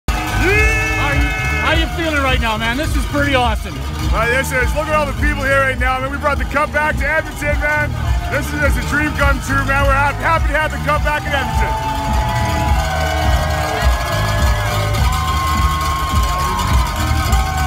630 CHED- Eskimos Grey Cup Celebration
Ryan King of the Eskimos and his reaction to great crowd at Sir Winston Churchill Square for the Grey Cup rally.